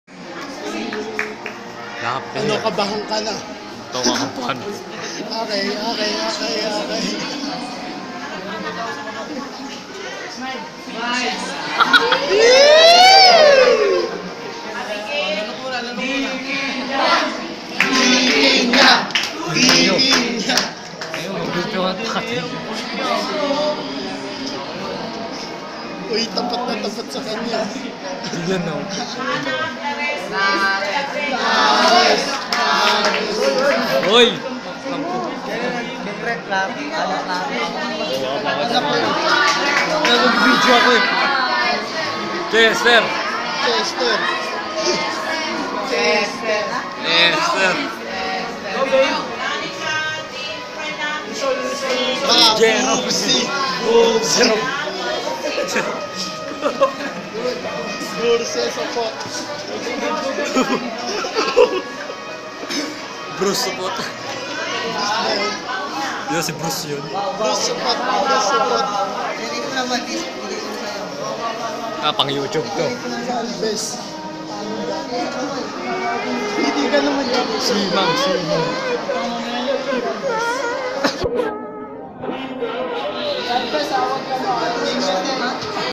Moving Up Ceremony of 10-BURGOS (FHS 2016-2017) clip 1
This clip shows the giving of moving up certificate (girls only)
 I'm really gonna miss them